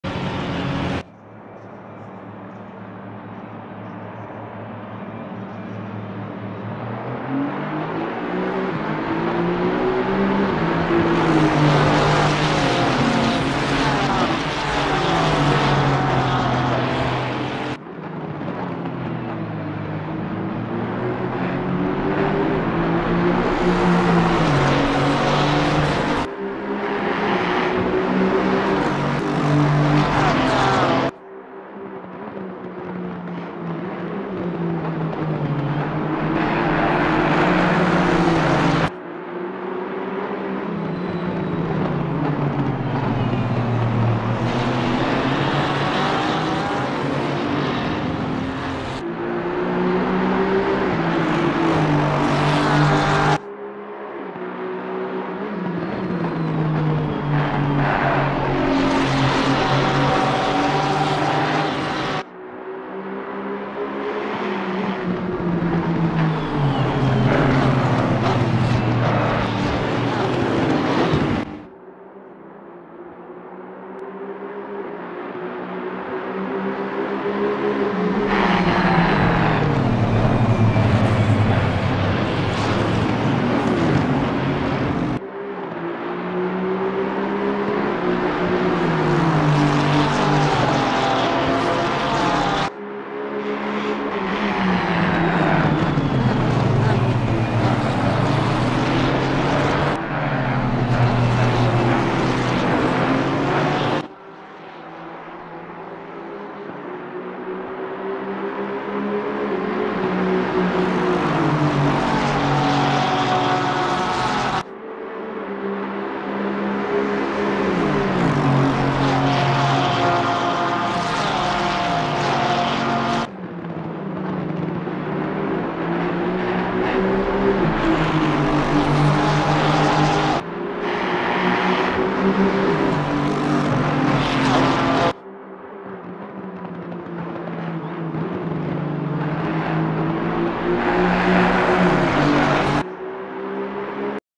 SRA D2 Imola Race Start - Aston Martin
Started on Pole for SRA D2 at Imola. #255 Aston Martin for TSE